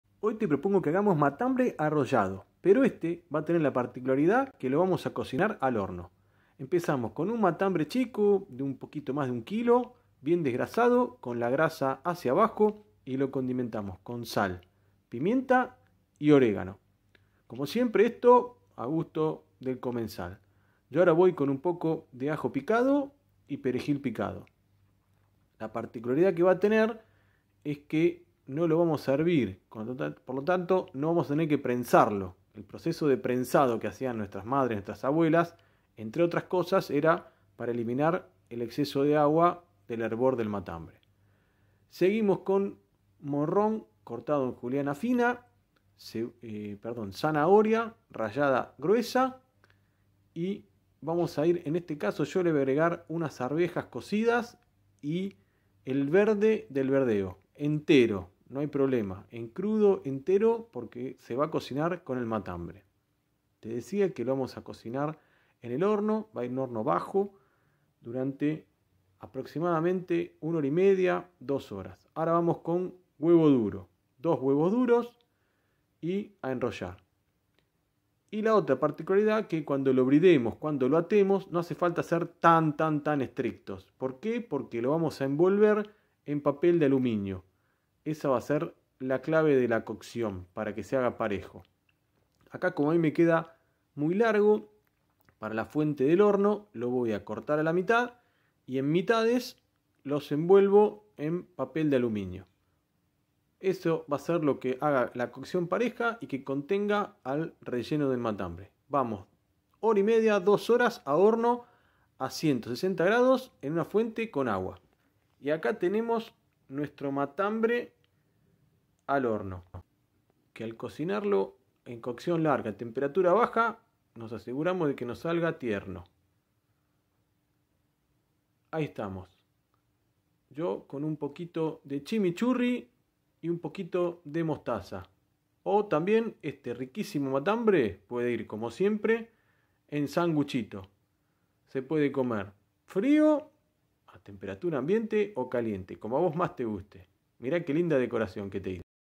Matambre arrollado al horno
Te muestro cómo hacer un riquísimo matambre arrollado al horno, que no necesitás prensarlo